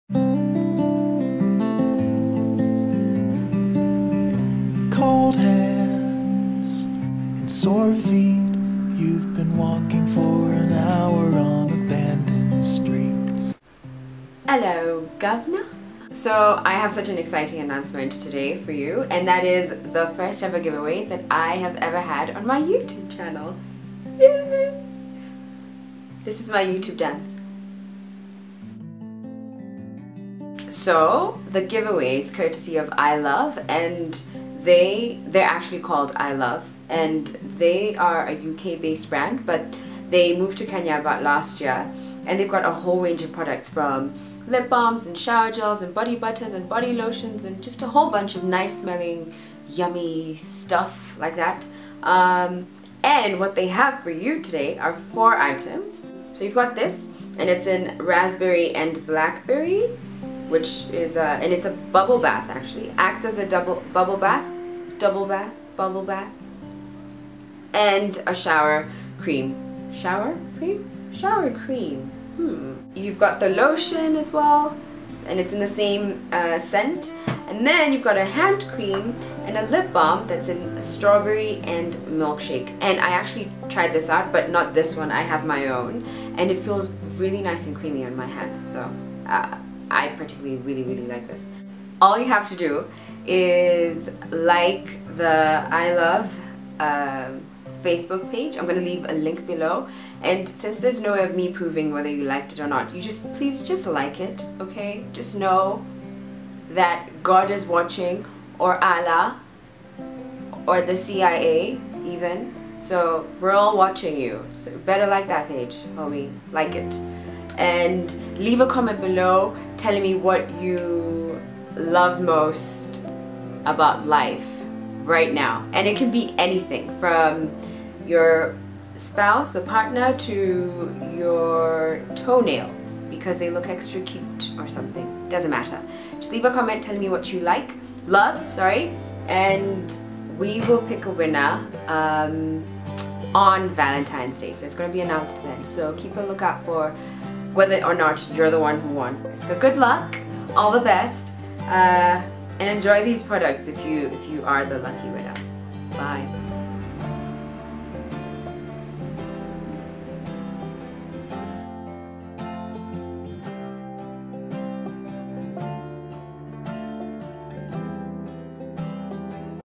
I Love... Valentine's Giveaway!!! {CLOSED}
Giveaway open ONLY to those residing in Kenya (sorry international viewers!)

AND

(2) Leave a comment telling me what you're loving at the moment!

Good Luck and Happy Valentine's Day :)